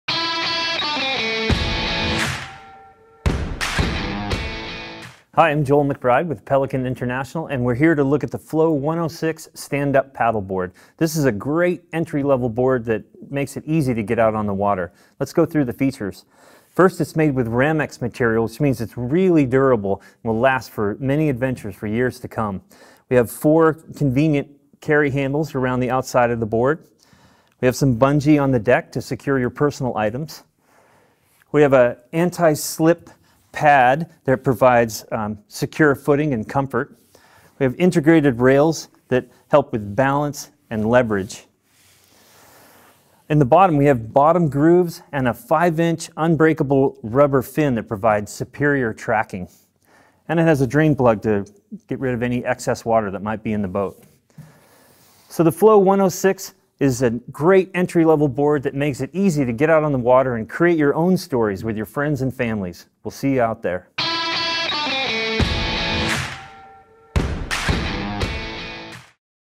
Pelican Flow (SUP)
The Flow 106 and 116 are steady and durable redesigned stand up paddleboards.

Discover the Joy of the Outdoors with Pelican